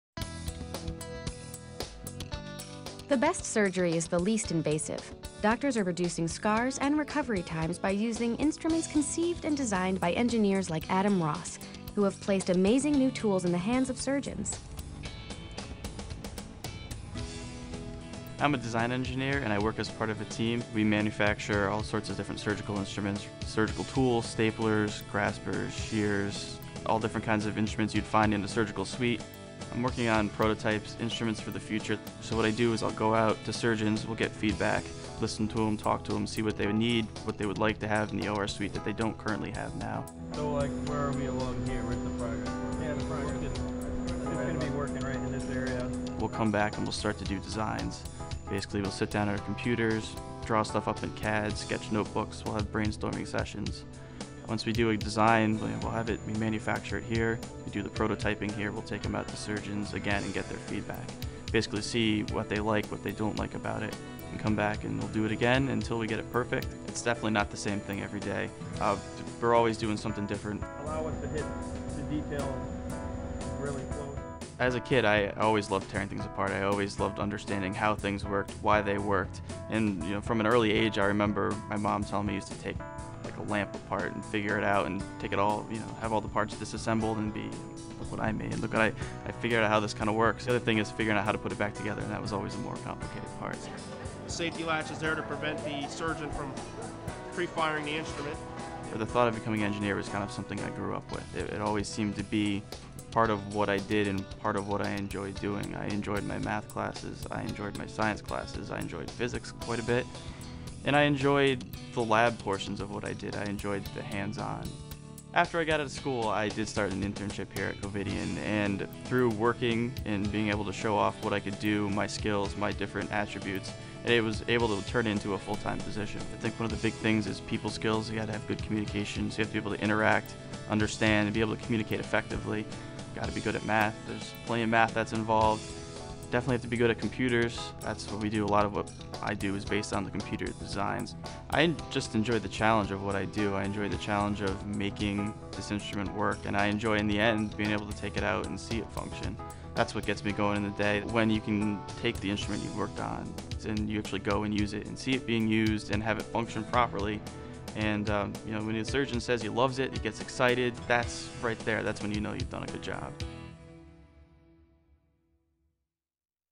Design Engineer
A typical day in the life of a young design engineer at a medical instrument facility.